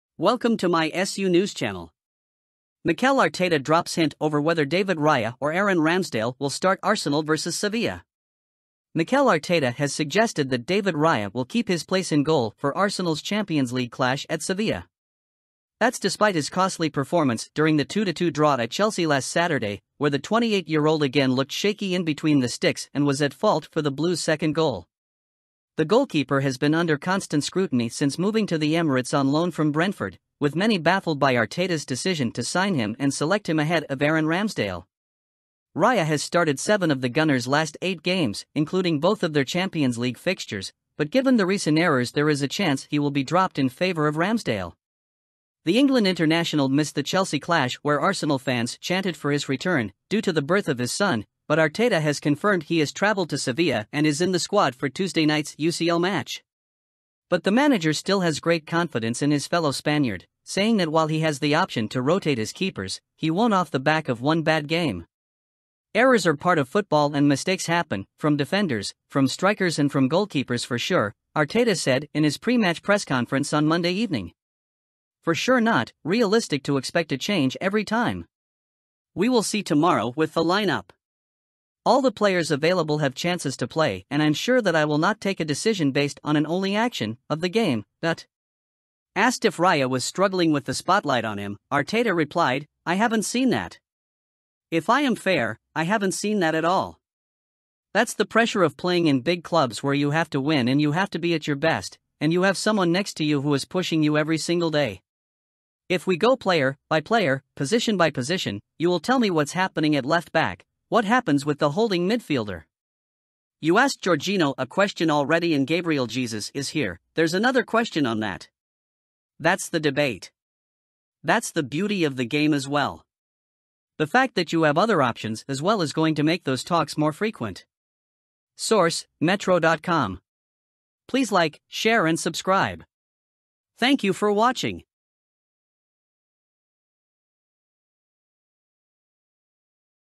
Mikel Arteta drops hint over whether David Raya or Aaron Ramsdale will start Arsenal vs Sevilla
News: Mikel Arteta drops hint over whether David Raya or Aaron Ramsdale will start Arsenal vs Sevilla, SUNews
1. Video Quality
2. Type of Breaking News and News Clip Content that you want.